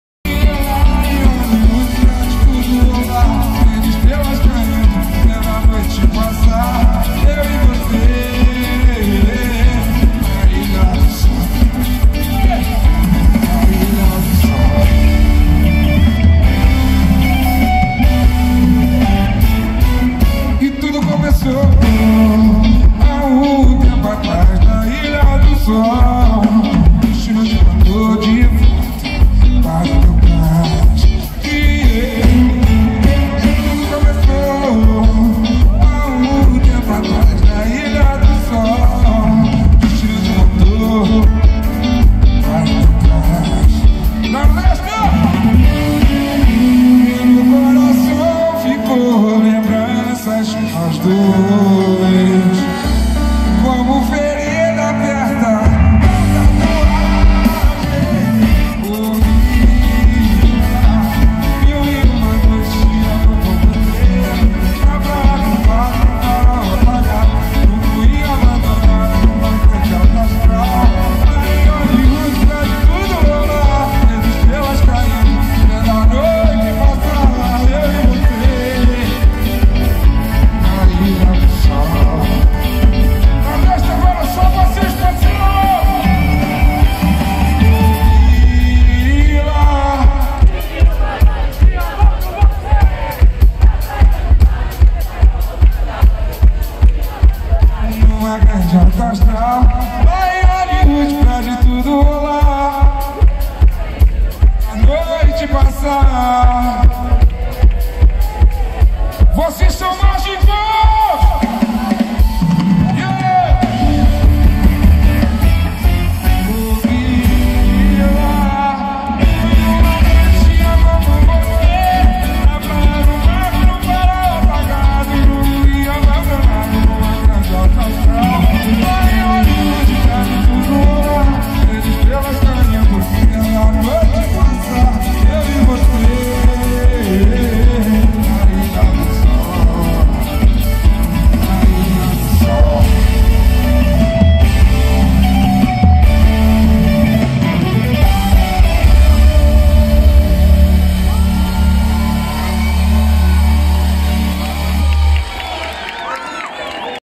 Maninho nas festas do Nordeste em São Miguel Açores 13.07.2023
Com muita animação assim se juntou uma multidão.